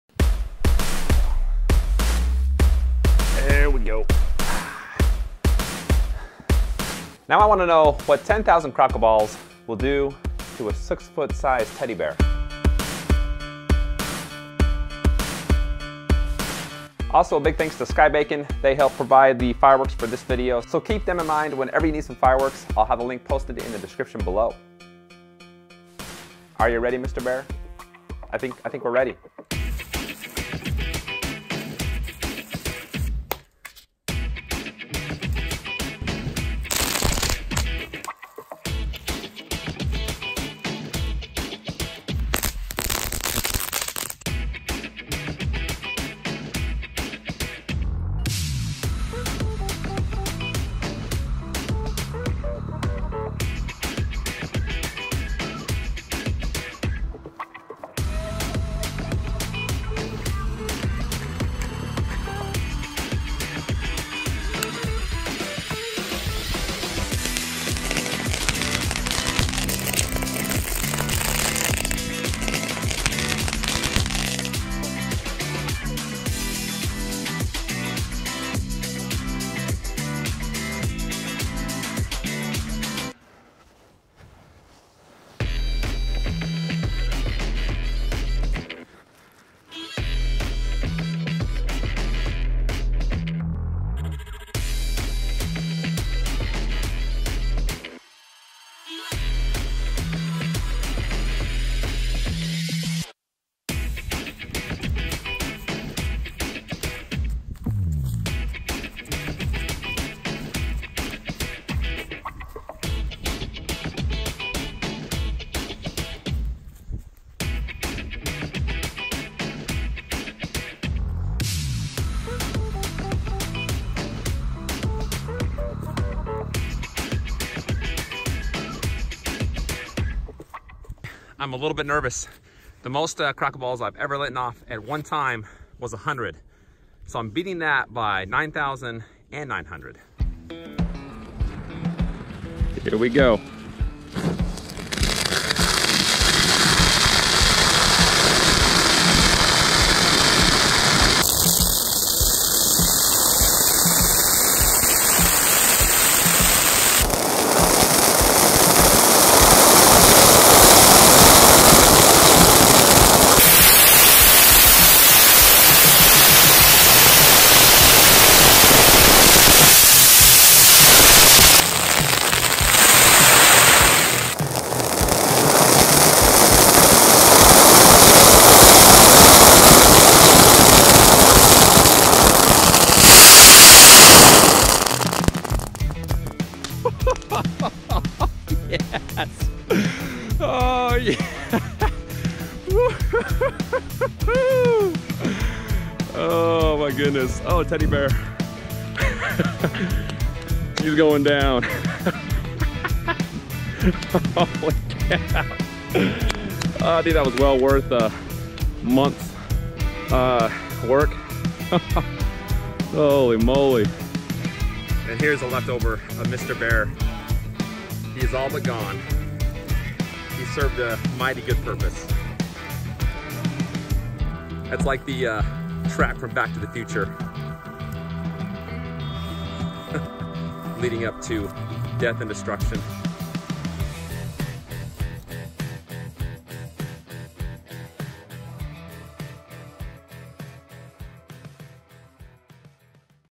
Poor Mr. Bear
A LOT of fireworks can do A LOT of damage to a teddy bear.